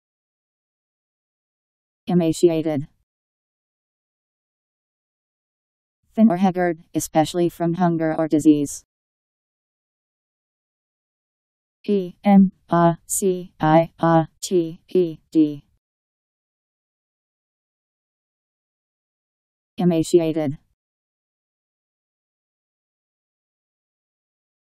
Emaciated Meaning
Video shows what emaciated means. Thin or haggard, especially from hunger or disease..  Emaciated Meaning. How to pronounce, definition audio dictionary. How to say emaciated. Powered by MaryTTS, Wiktionary